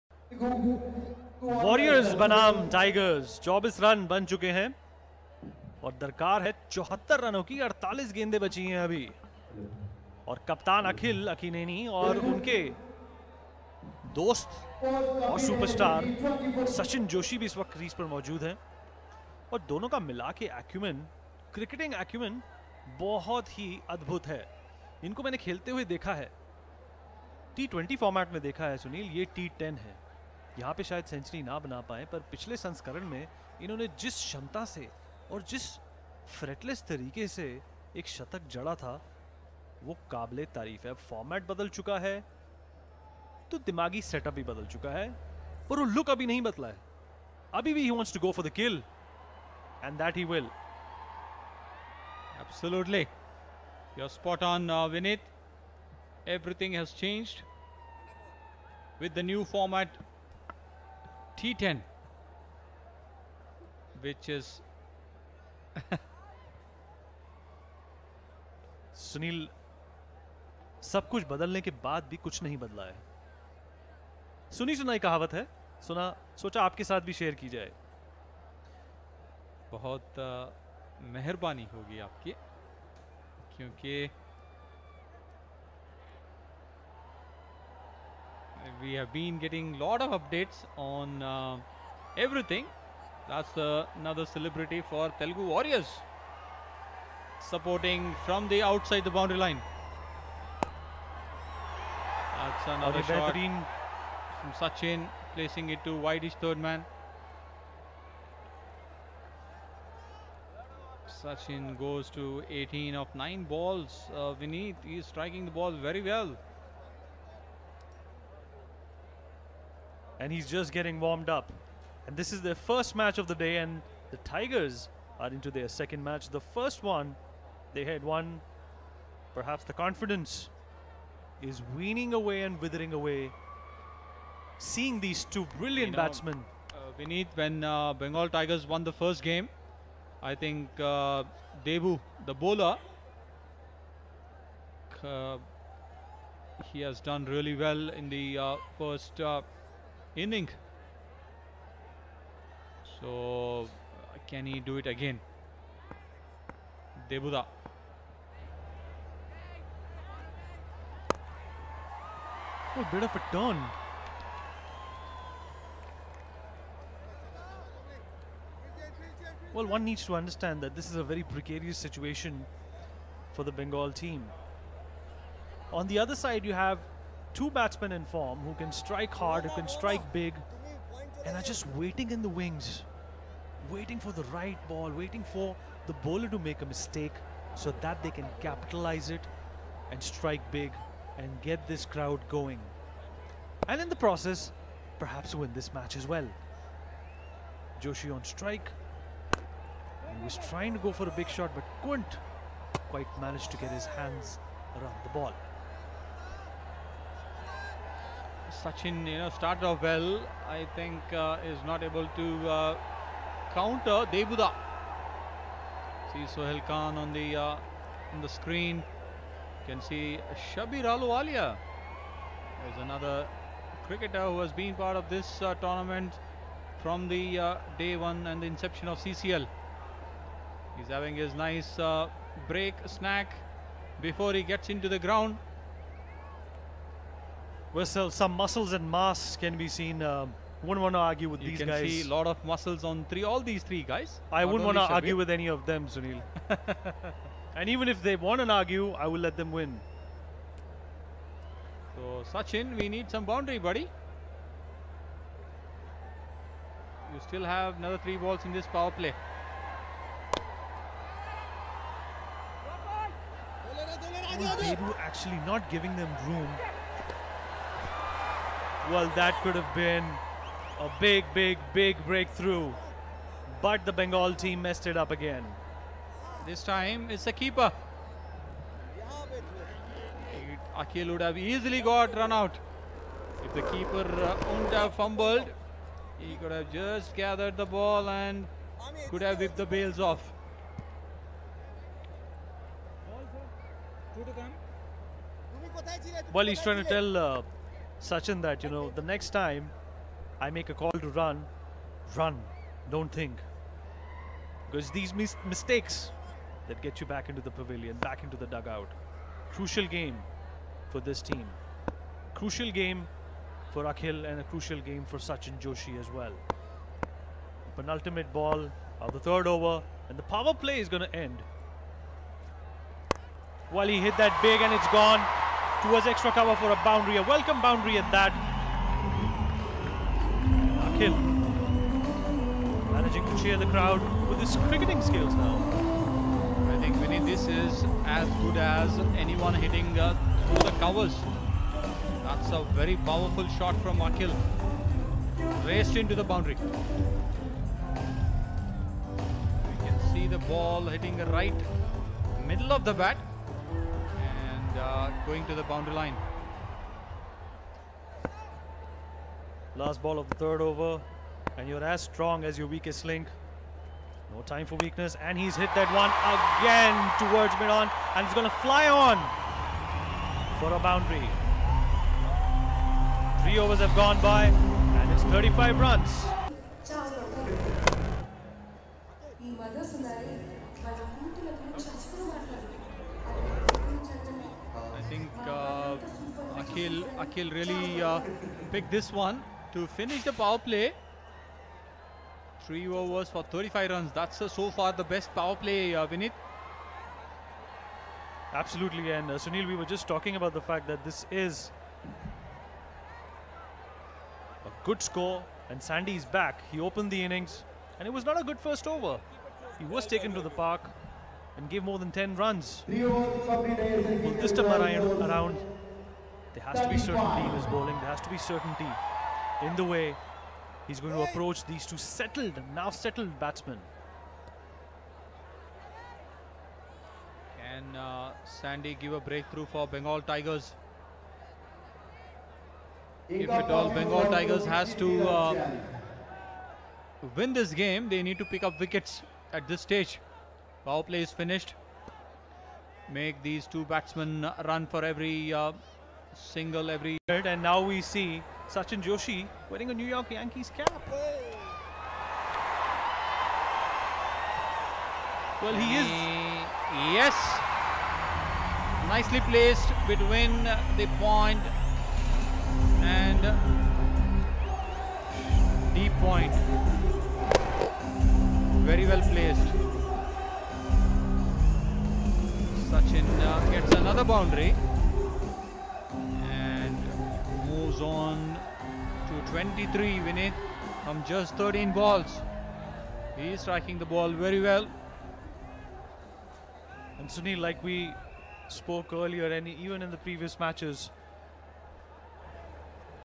Telugu Warriors Supporters Enjoying Akhil Akkineni's Powerful Shots Against Bengal Tigers
Telugu Warriors Supporters Enjoying Akhil Akkineni's Powerful Shots Against Bengal Tigers In Celebrity Cricket.